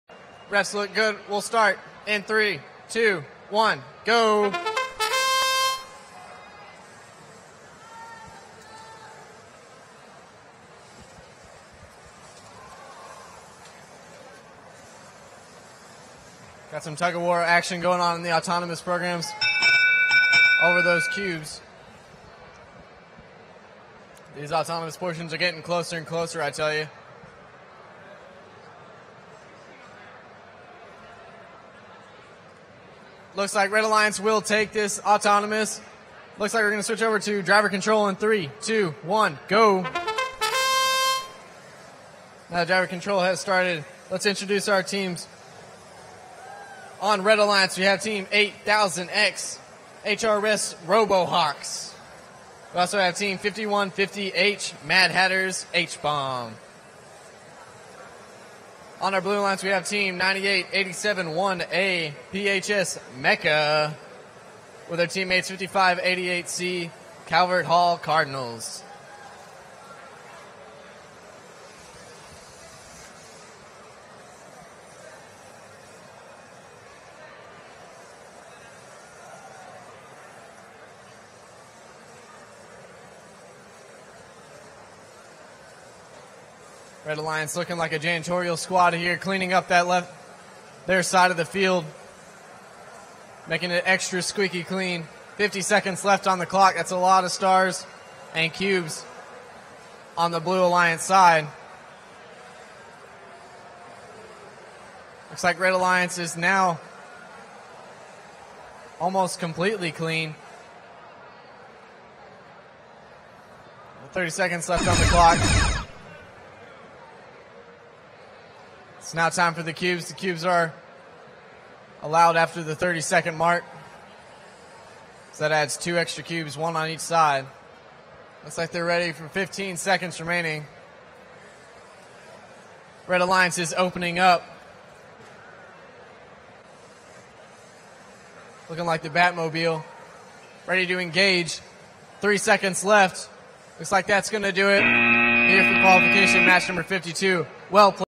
2017 VRC Arts Q52 - 8000X 5150H vs 98871A 5588C - 42 to 4
VEX Worlds 2017 - VRC High School - Arts Division - Qualifiers Match 52

Red - Score 42:
8000X - HRS Robohawks (Head-Royce School, Oakland, California, United States)

5150H - Mad Hatters - H-Bomb (Danbury High School, Danbury, Connecticut, United States)

Blue - Score 4:
98871A - PHS MECHA (PBTISD, Pecos, Texas, United States)
||  Arts Rank:86 . Wins:2 . Loses:8 . WP:4 . AP:16 . SP:135

5588C - Calvert Hall Cardinals (Calvert Hall College High School, Towson, Maryland, United States)
||  Arts Rank:61 . Wins:4 . Loses:6 . WP:8 . AP:20 . SP:124